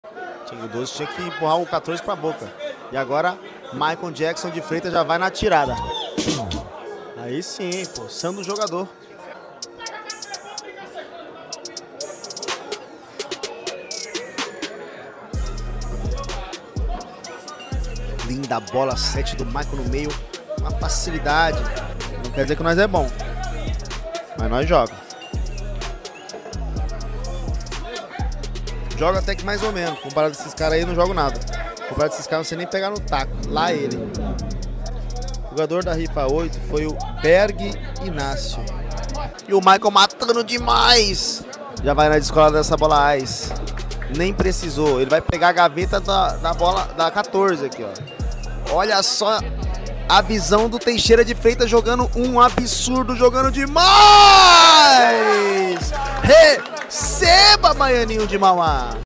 Maycon jogando muito contra baianinho de mauá 🔥🎱
Salve Salve Sinuqueiros!
Deixa um comentário aí em baixo e avalie e conteúdo!
Face/Insta: @omundodasinuca
Meu Insta: @phsinuca
Grato a Todos!